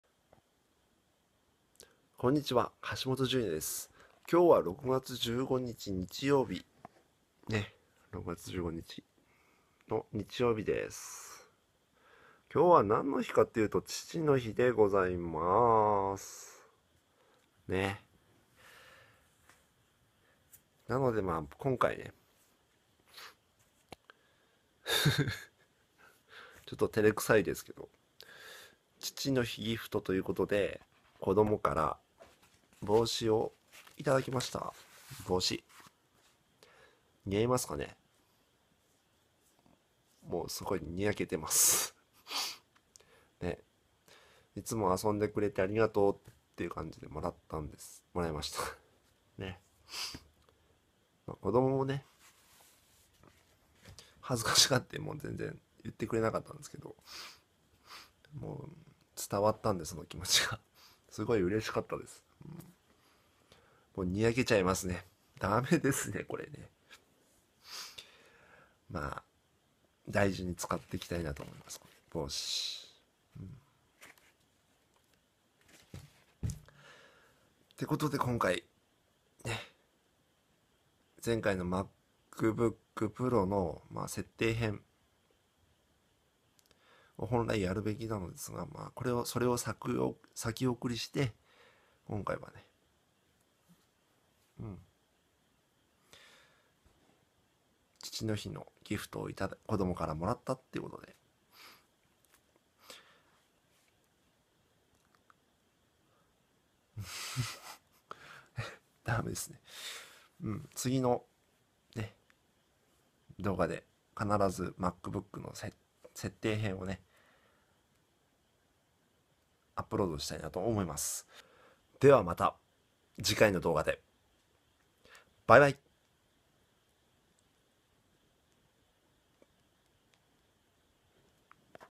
初の父の日 ギフト！ プレゼントもらいました＾＾
初めて子供逹と、妻から父の日のプレゼントいただきました^o^
ってかこんなに早くもらえるなんて思ってもなかったです。
かなり嬉しい＾＾

やっぱり照れくさかったです。(^^;;
ありがとう
これからもいっぱい色んなところ行こうな^o^